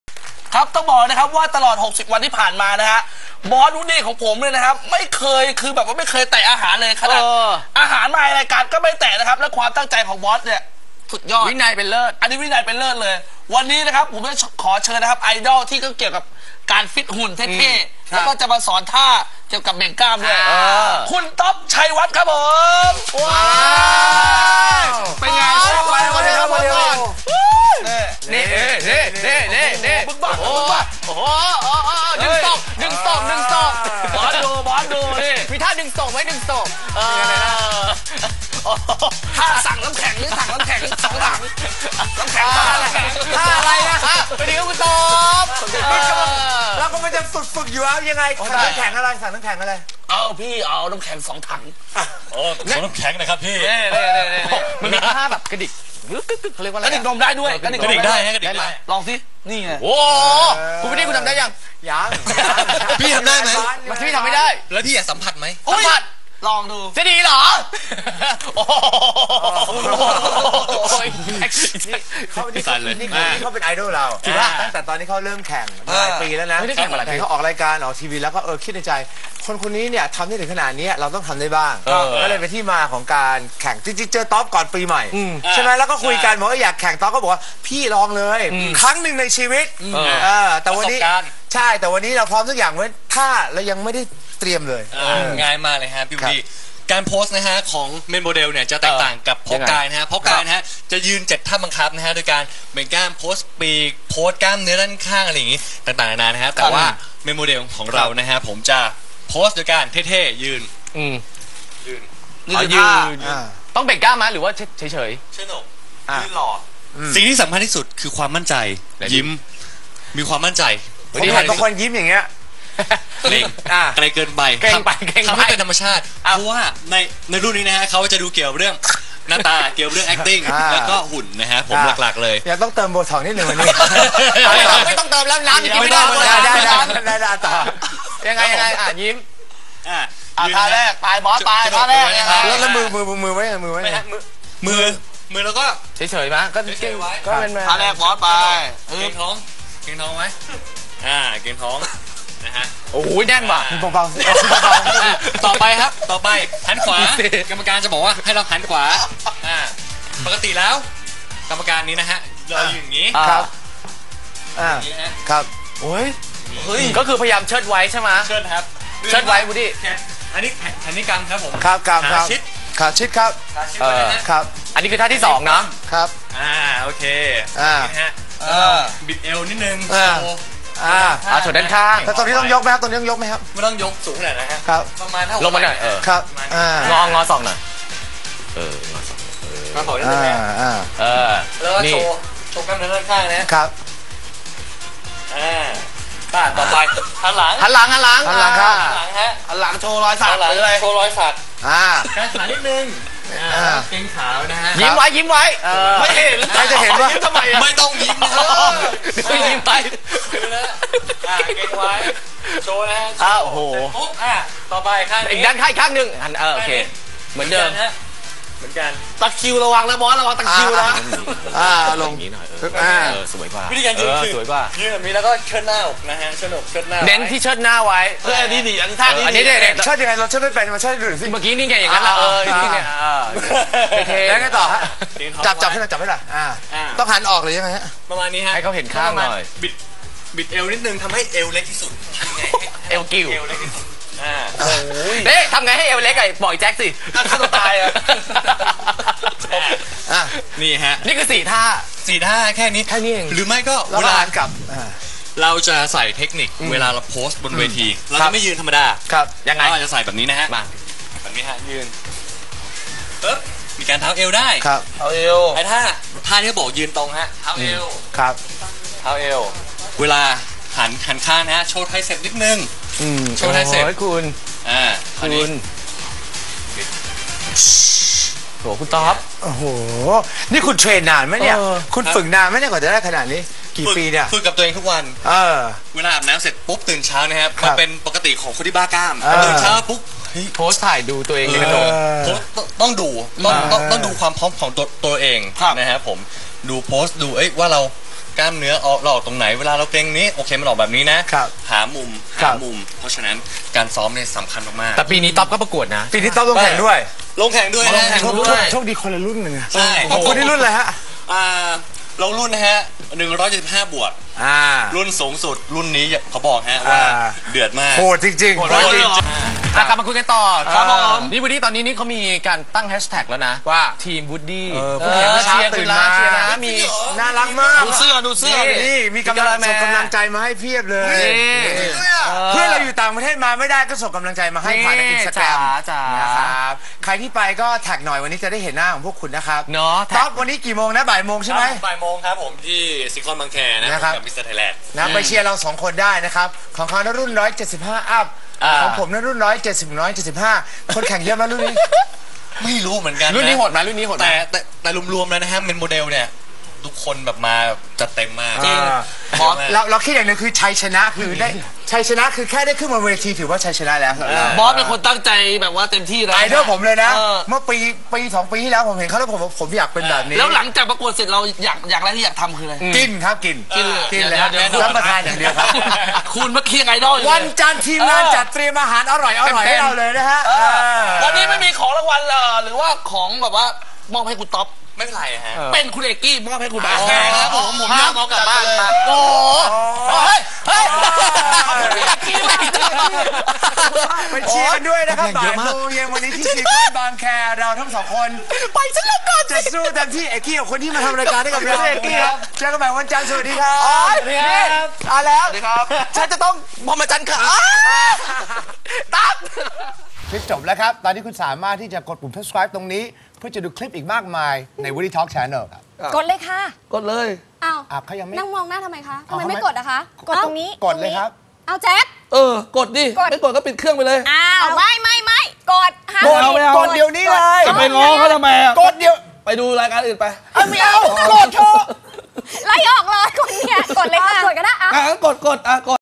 ต๊อบ สอน วู้ดดี้ ก่อนลงแข่งเพาะกาย Mr.Thailand
ตื่นเต้นกันสุดๆก่อนลงแข่ง ต๊อบเลยขออาสามาฝึกให้ในระยะเวลากระชั้นชิด เพื่อเพิ่มความมั่นใจ ก่อนลงแข่งเพาะกาย Mr.Thailand!!